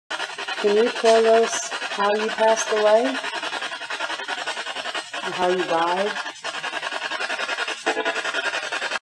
Spirit tells how he passed away.
Investigator ask Steven how he passed away, how he died? Male voice replies 'Heart'. Please comment if you hear anything else?